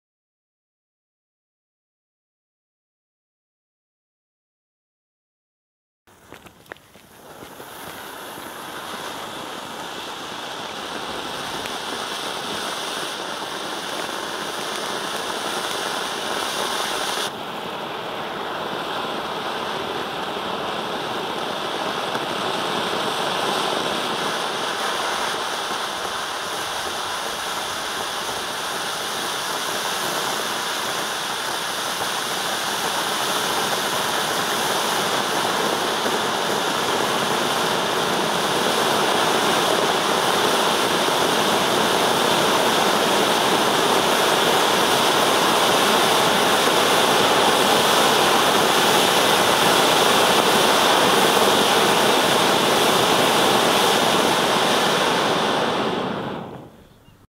Goldflimmervulkan 45sec | Läubli
Toller Goldvulkan aus dem Hause Läubli. Ich mag die mittleren Vulkangrössen da die für den Preis immer recht viel bieten.